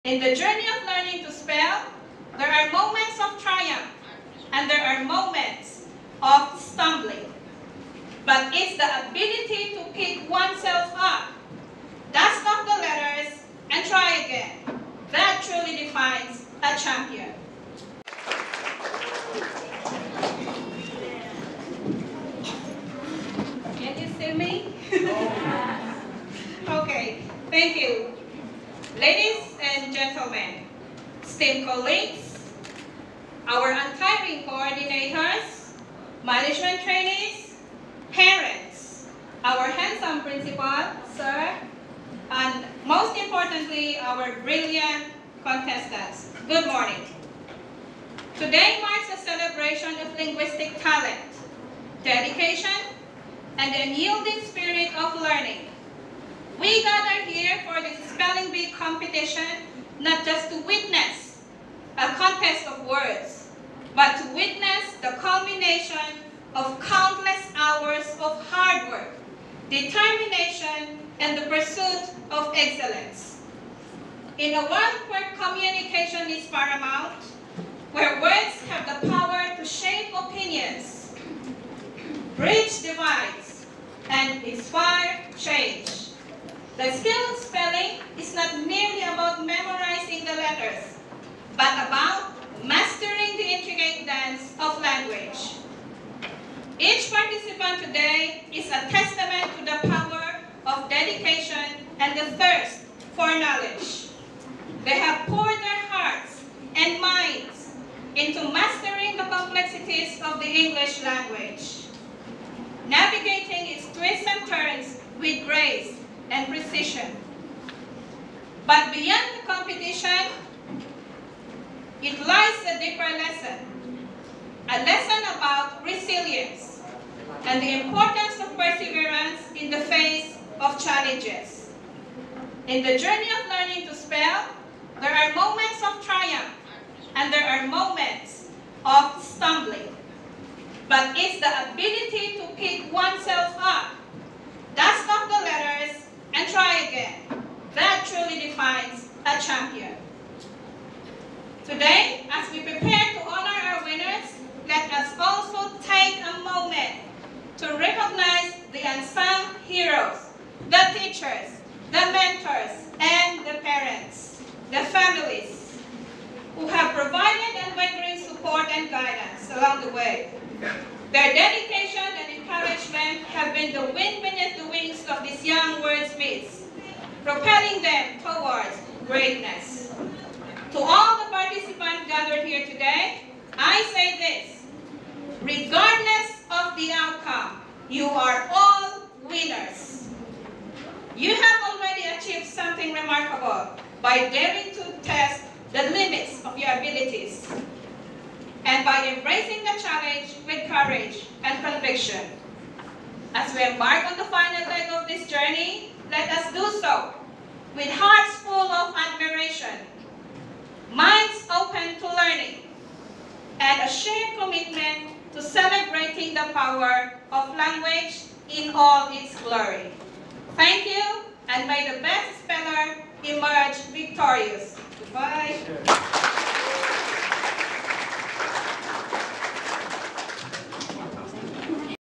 honored 2B the Bee Master 4 the final spelling bee competition and a judge representative speaker.
I was honored to be the Bee Master for the final spelling bee competition and was assigned as the judge representative speaker. I prepared and practiced my speech, though I couldn't memorize it due to being busy writing sample sentences. The delivery wasn't perfect, but I'm grateful for the opportunity. This was my second time speaking in front of contestants, parents, the management team, and colleagues. It was a great chance to test my bravery and face challenges head-on. Despite the imperfections, I'm proud of my effort and the experience I gained. My name is Ailhy Glea 🌿😍